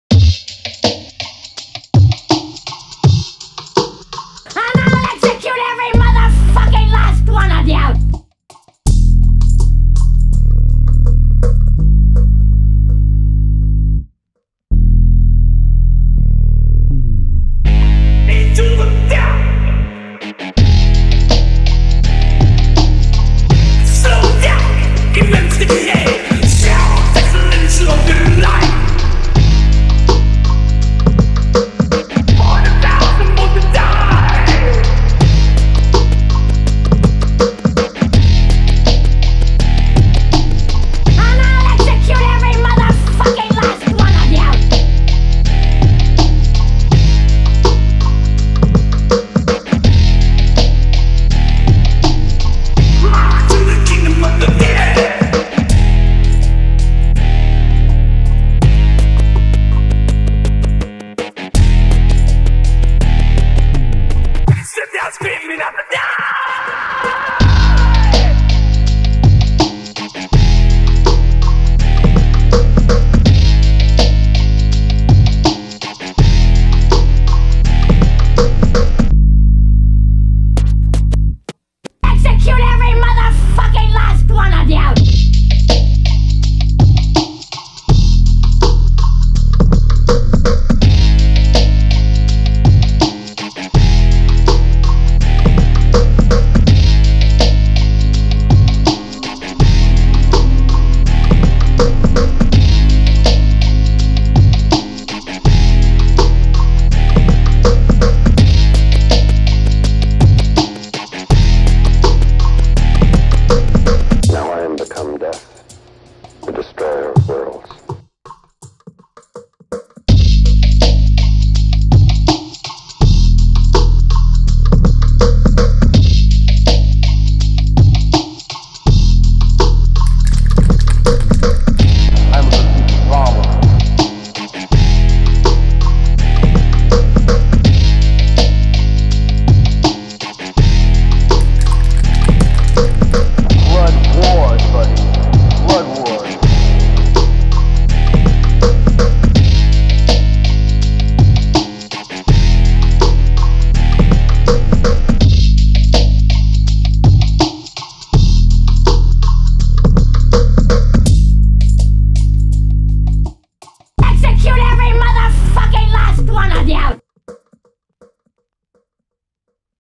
Graffiti Art : FEMA * Death Camp / Time-lapse
Check out this FEMA time-lapse. Follow along to see how to rock graffiti burners.